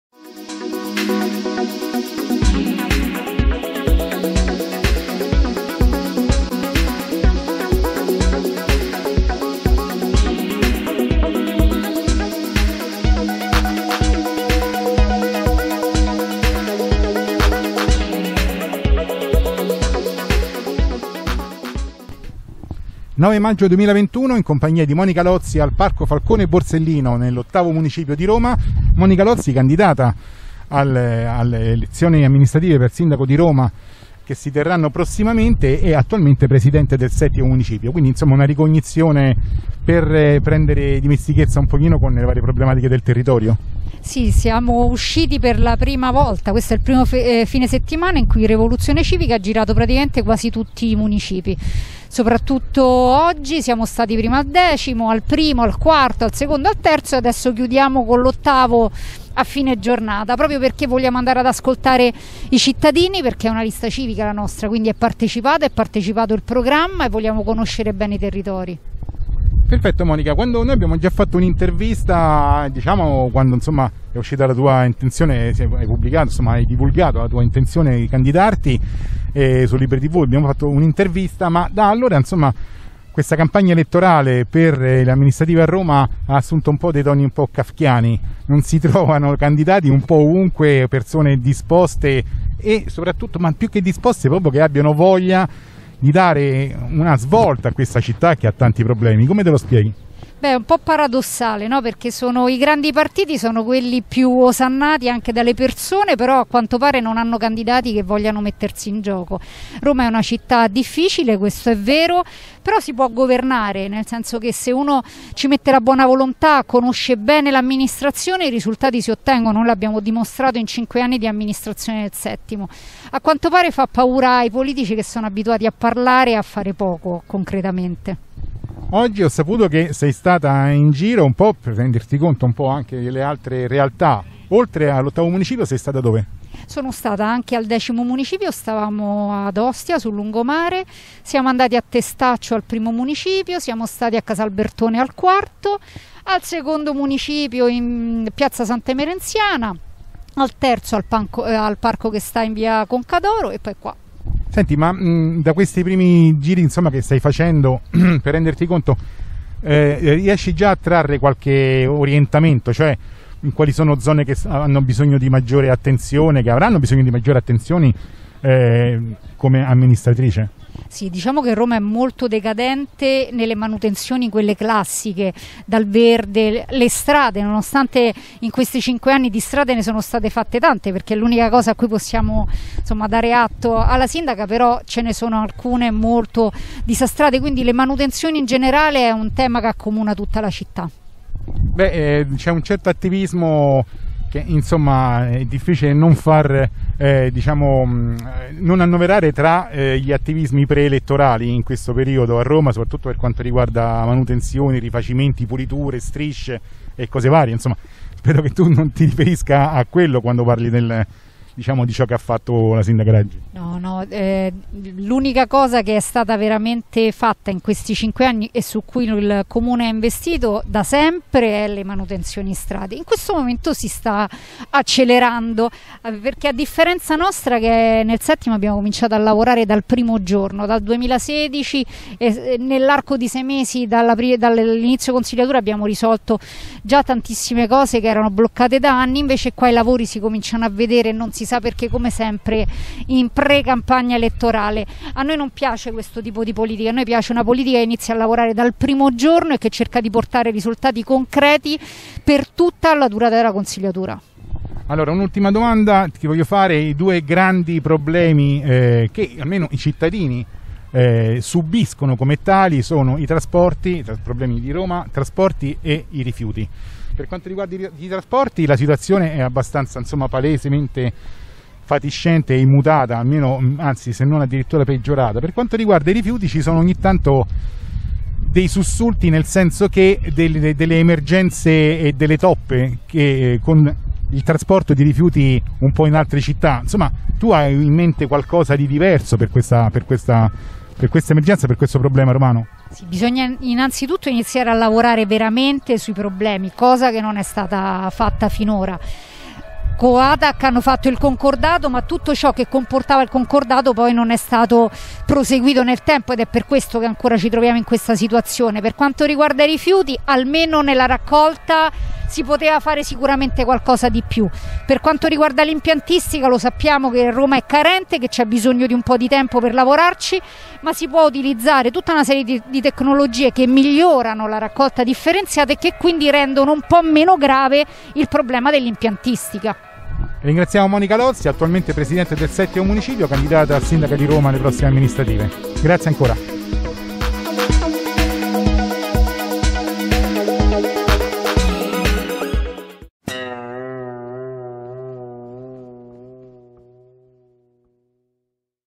Monica Lozzi, candidata Sindaca di Roma per ЯEvoluzione Civica - Campidoglio 2021
Monica Lozzi, candidata Sindaca di Roma per ЯEvoluzione Civica
9 maggio 2021 - A cura di Gianni Colacione